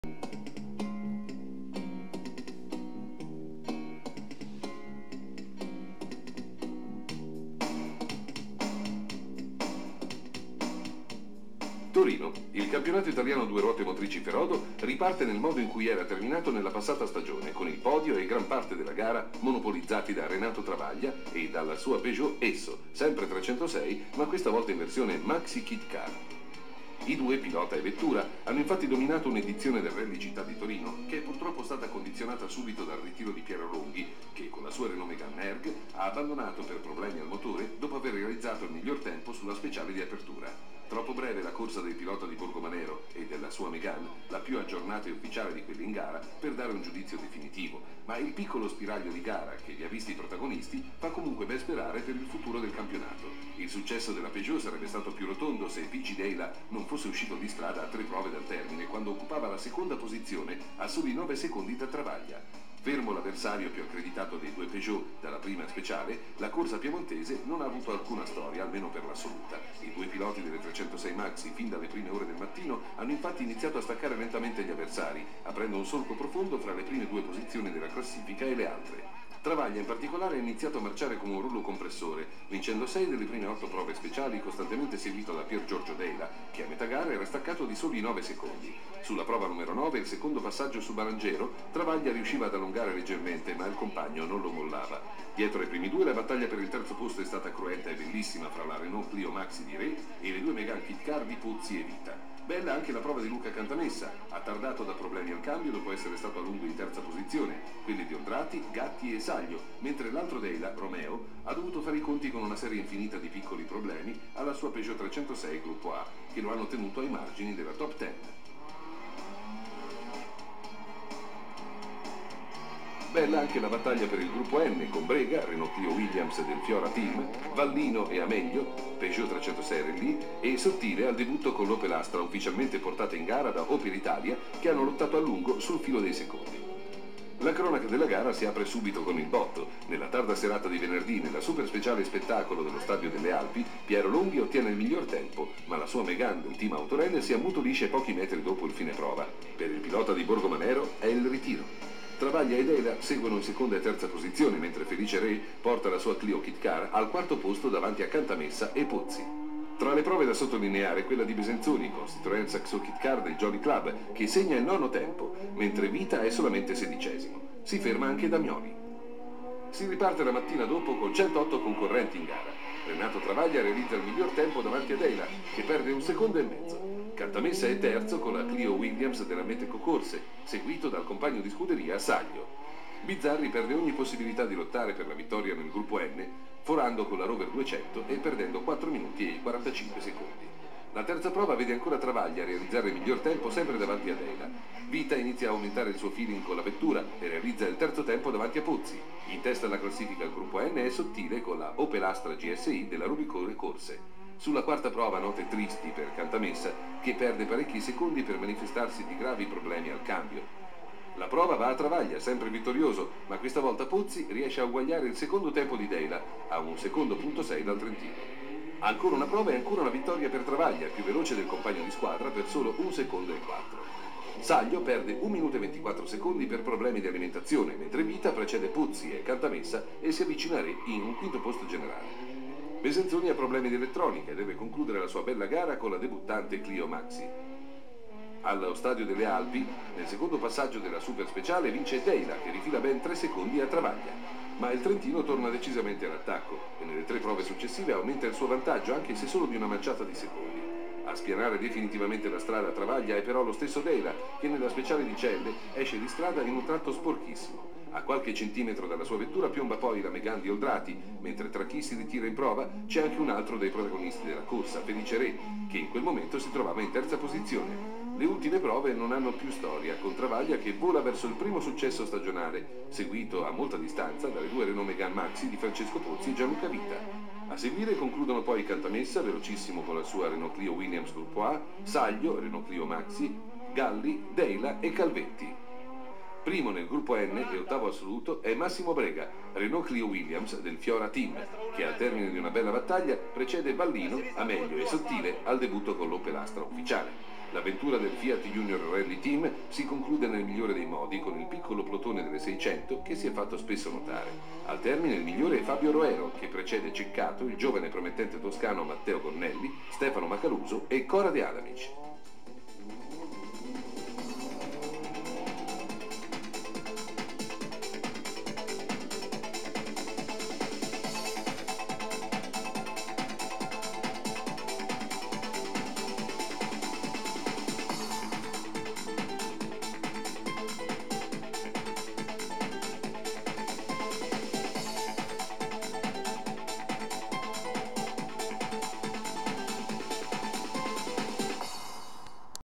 14° Rally Città di Torino 1999 [RallySprint]
14° Rally Città di Torino 1999
Campionato Italiano 2 Ruote Motrici Rallyes 1999

1° Travaglia/Zanella - Peugeot 306 Maxi
2° Pozzi/Foggiato - Renault Megane Maxi
3° Vita/Agnese - Renault Megane Maxi
4° Cantamessa/Capolongo - Renault Clio Williams Gr.A
5° Saglio/Fatichi - Renault Clio Maxi